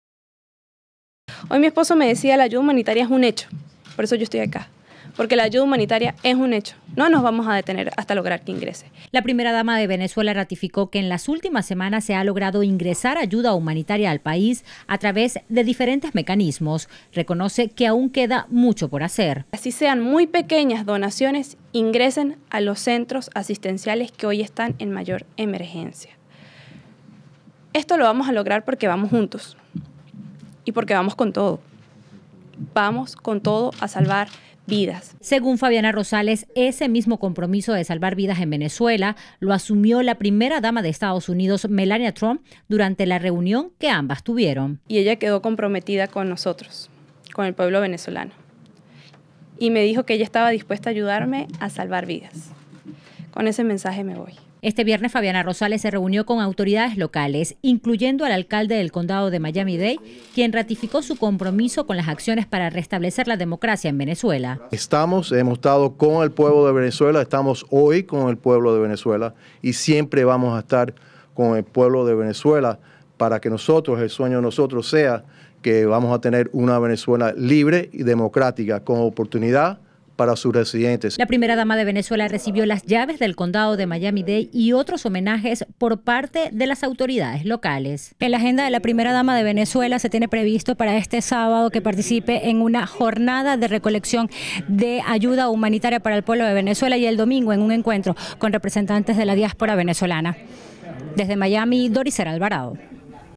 Primera Dama Fabiana Rosales realiza gira para hablar de la crisis en Venezuela
La primera dama de Venezuela, Fabiana Rosales, comenzó su gira por el sur del estado de la Florida.  Este viernes sostuvo reuniones con diferentes autoridades locales para hablar sobre la ayuda humanitaria en Venezuela, aseguró que el ingreso de los donativos ya es un hecho.